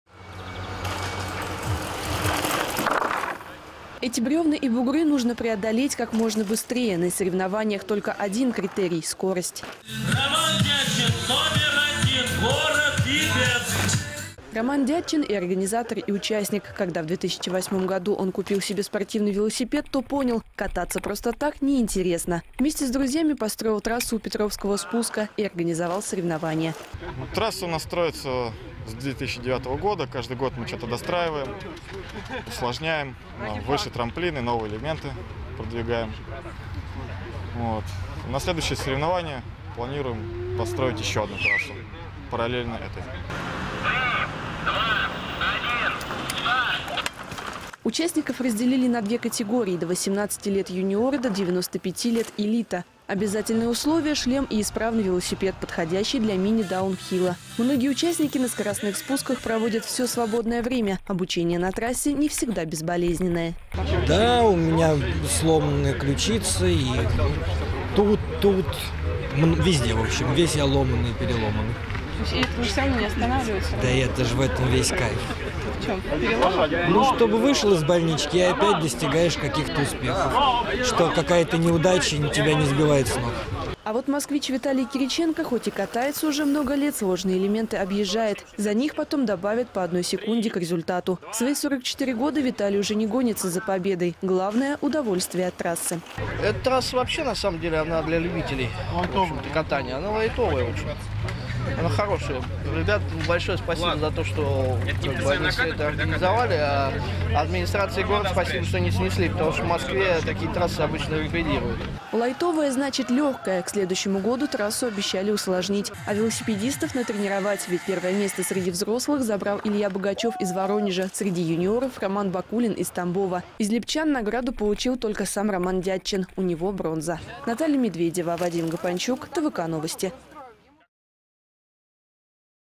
В Липецке прошли соревнования по скоростному спуску на велосипеде
Кто лучше всех катается с горки на велосипеде. Экстрималы из разных городов России приехали в Липецк, чтобы это выяснить. Мини даун хилл — это скоростной спуск на велосипеде с препятствиями,  направление в Липецке   новое, но увлеченных им уже много. Попробовать липецкую трассу приехали 25 участников самого разного возраста. Наталья Медведева следила за соревнованиями.